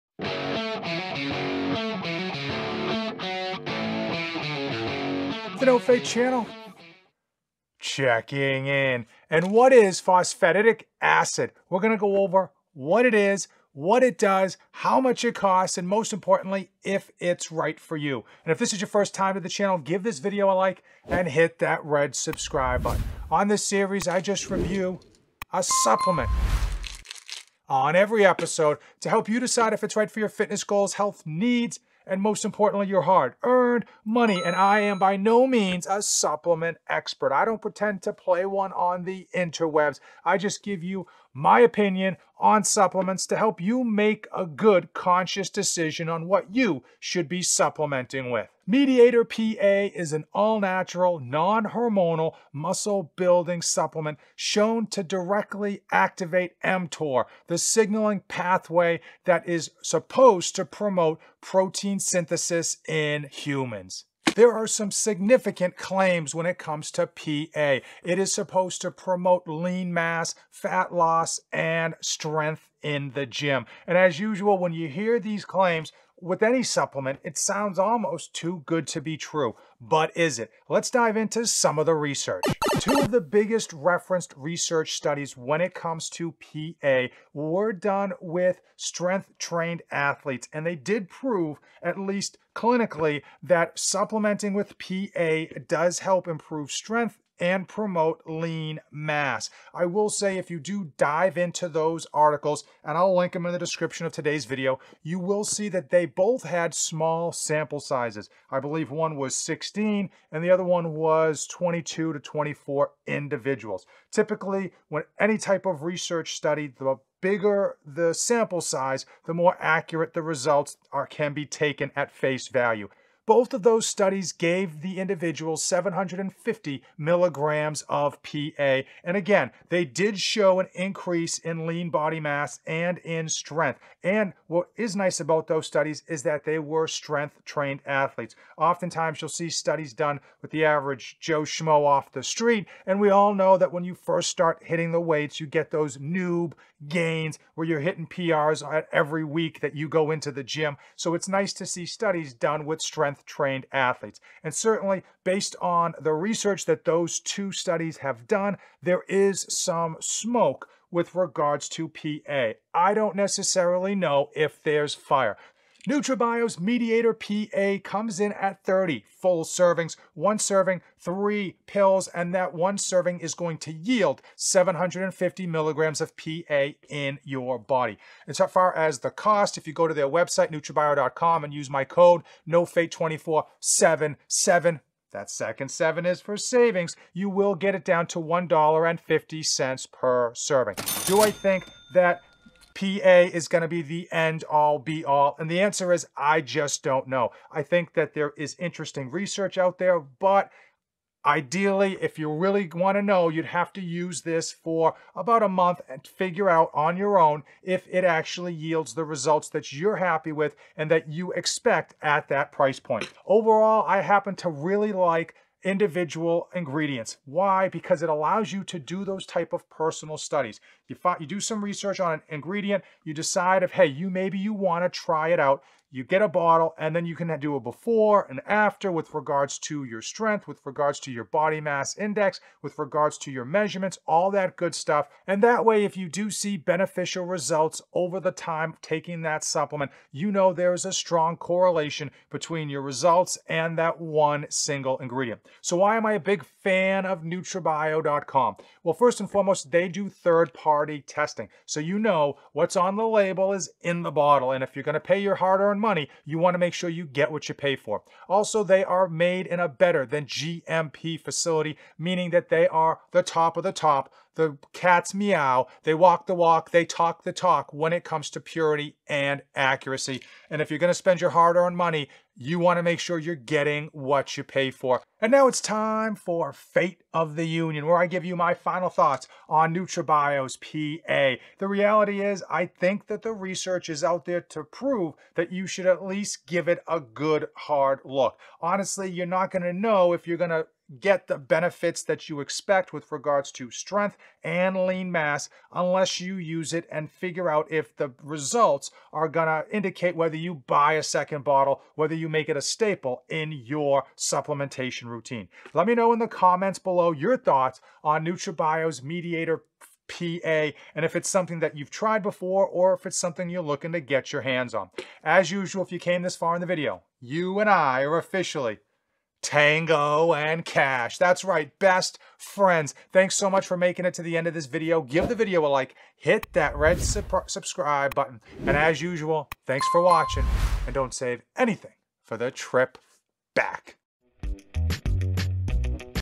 Does Phosphatidic Acid WORK!?
Mediator PA, Phosphatidic ACID – Will it get you lean mass, more strength and help with fat loss?  I break down what phosphatidic acid is, what the claims behind it are, and what the studies show.

Phosphatidic Acid
• 500mg Mediator ® PA per capsule
• Enhances lean mass gains*
• Supports incresed strength*
• Encourages fat loss & body recomposition*
• Combats muscle breakdown*
• Promotes muscle recovery*

Muscle growth and recovery is ultimately a balance of protein synthesis vs protein breakdown.
Phosphatidic acid is natural and non-hormonal.
1. Hoffman JR, Stout JR, Williams DR, et al. Efficacy of phosphatidic acid ingestion on lean body mass, muscle thickness and strength gains in resistance-trained men. Journal of the International Society of Sports Nutrition. 2012;9:47. doi:10.1186/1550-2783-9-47.
2. Joy JM, Lowery RP, Dudeck JE, De-Souza EO, Jager R, McCleary SA, Wilson SMC, Purpura M, Wilson JM. Phosphatidic Acid Supplementation Increases Skeletal Muscle Hypertrophy and Strength. Poster presentation at the ISSN Conference 2013
3. Bond, P. (2017). Phosphatidic acid : biosynthesis , pharmacokinetics , mechanisms of action and effect on strength and body composition in resistance-trained individuals, 1–9.
4. Purpura M, Jager R, Joy JM, Lowery RP, Moore JD, Wilson JM. Effect of Oral Administration of Soy-Derived Phosphatidic ACid on Concentrations of Phosphatidic Acid and lyso-Phosphatidic Acid Molecular Species in Human Plasma. Poster Presentation at the ISSN Conference 2013.
5. Dudeck JE, Joy JM, Lowery RP, De Souza EO, Jager R, McCleary SA, Wilson SMC, Purpur M, Wilson JM. Safety of Soy-Derived Phosphatidic Acid Supplementation in Healthy Young Males. Poster presentation at the ISSN Conference 2013